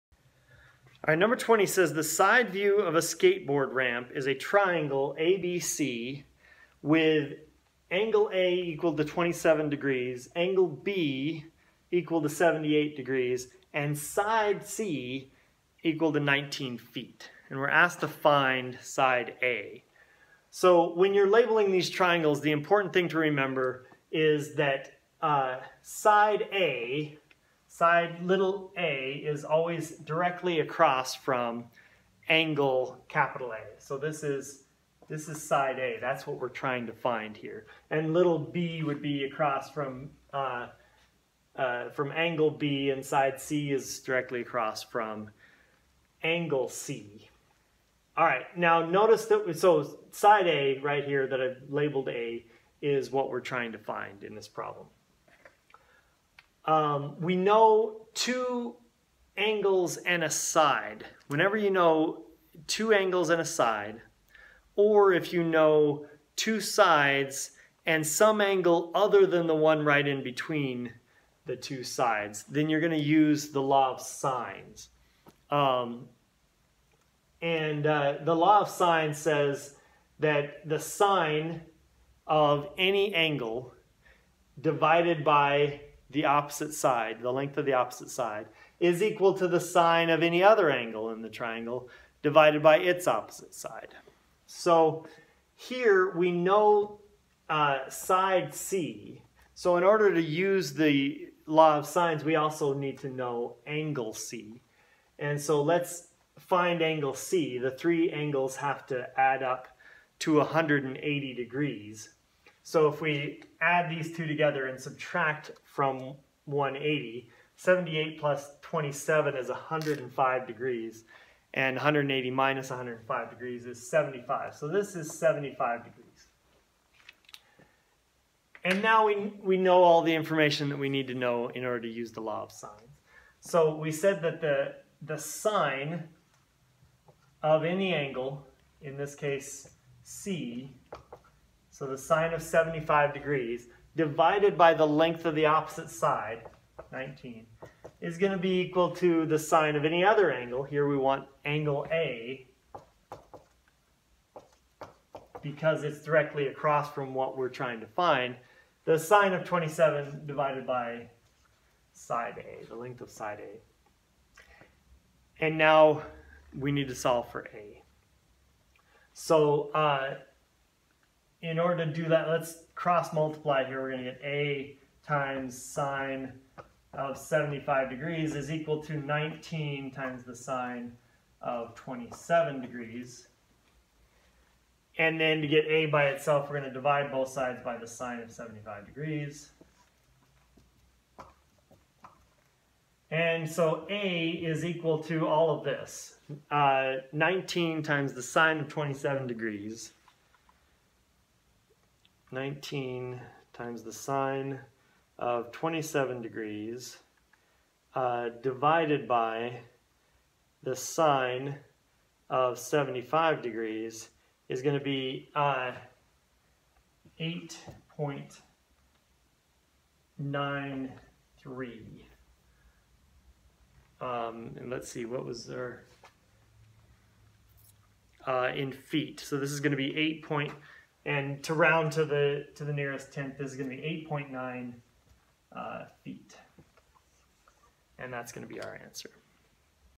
Precalculus Final Review (Trigonometry) Problem 20
The side view of a skateboard ramp is a triangle ABC with A = 27 degrees, B = 78 degrees, and c = 19 feet.  Find a.